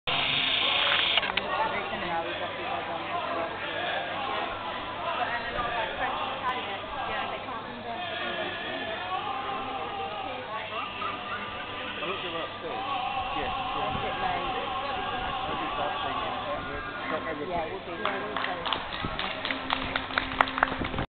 Chester city centre opera singer and "friend"
An opera singer in chester city centre and conductor.